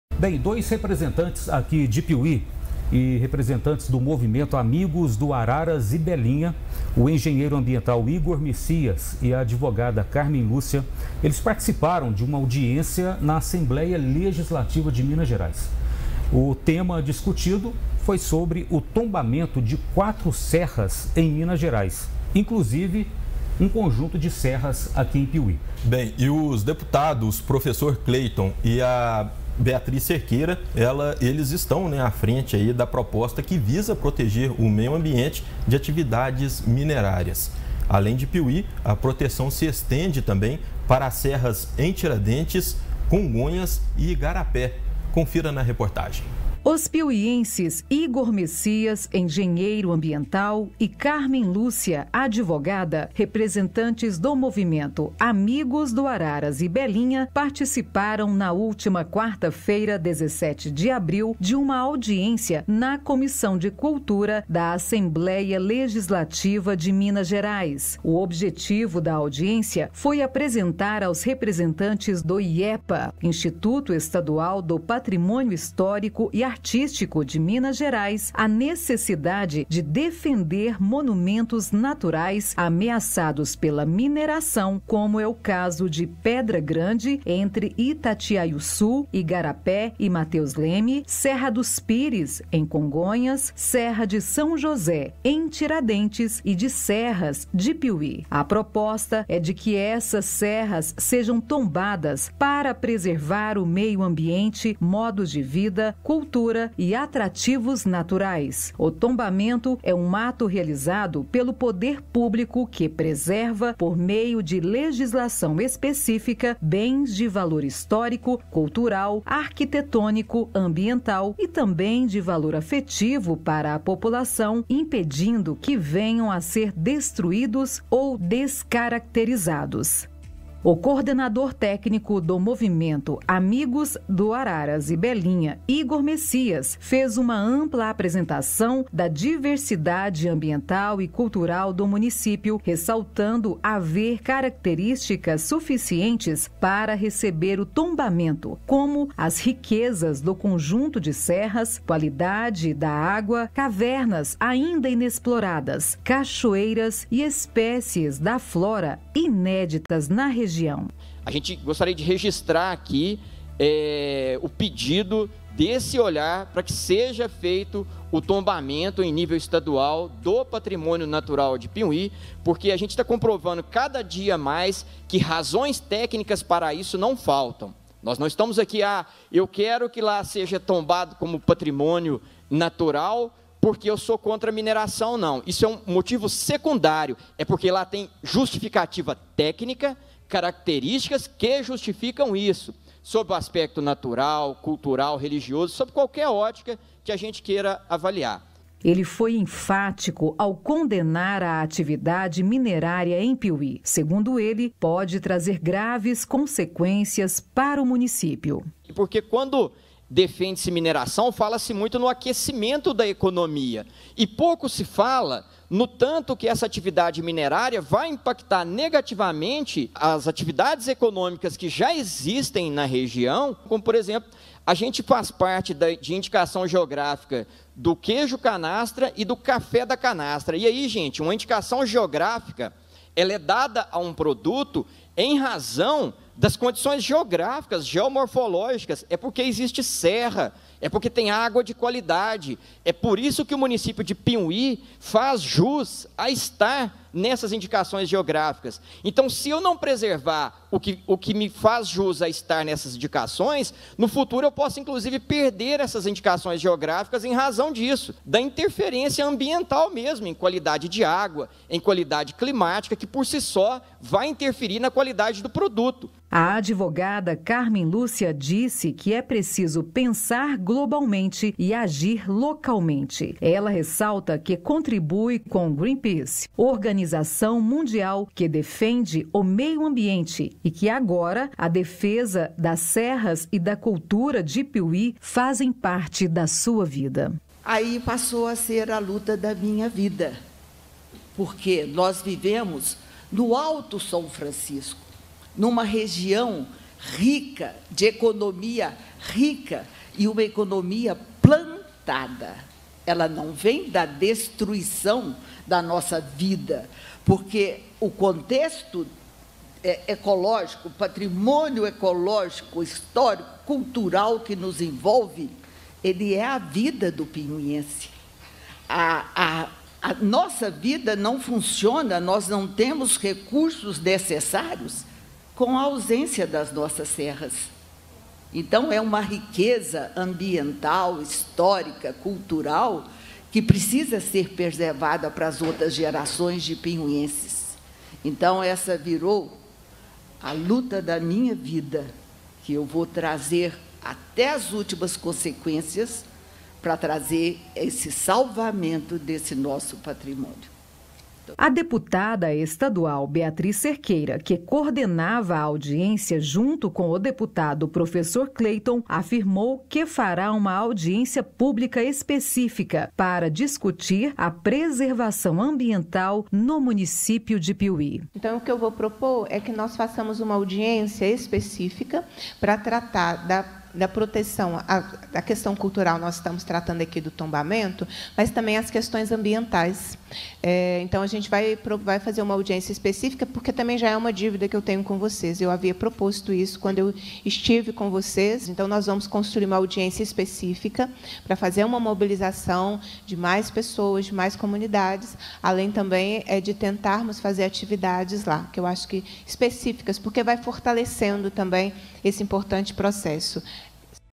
REPRESENTANTES DO MOVIMENTO AMIGOS DO ARARAS E BELINHA PARTICIPAM DE EVENTO NA ASSEMBLEIA DE MINAS
| PIUMHI |

Dois representantes do movimento Amigos do Araras e Belinha, o engenheiro ambiental Igor Messias, e a advogada Carmen Lúcia, participaram de uma audiência na Assembleia sobre tombamento de quatro serras em Minas, inclusive serras em Piumhi. Acompanhe todas as informações na matéria: